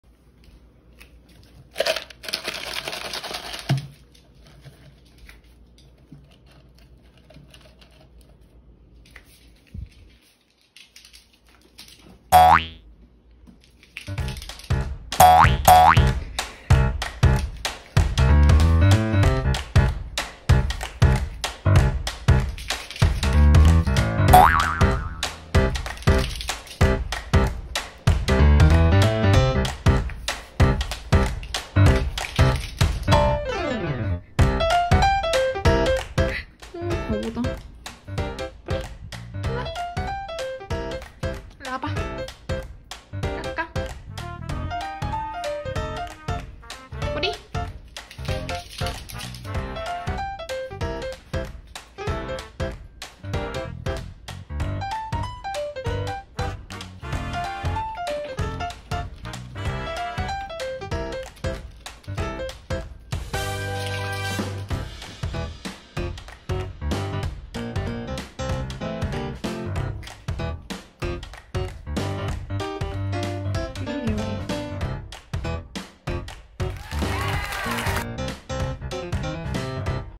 토끼처럼 뛰는 미니핀
간식따라 폴짝폴짝 이불위를 왜 뛰어다닐까요?
다른 미니핀 친구들도 이런가요..? 귀여워 죽겠다